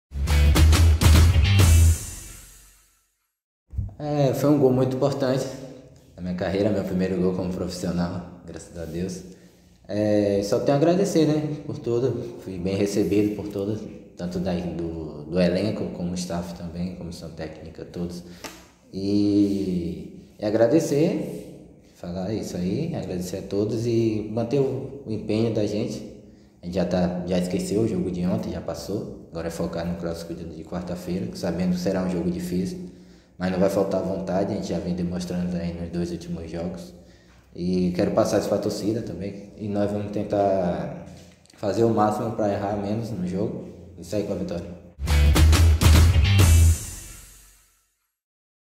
Volante Alê Santos fala sobre o gol marcado diante do Concórdia, o seu primeiro como profissional
Imagens: TV Figueira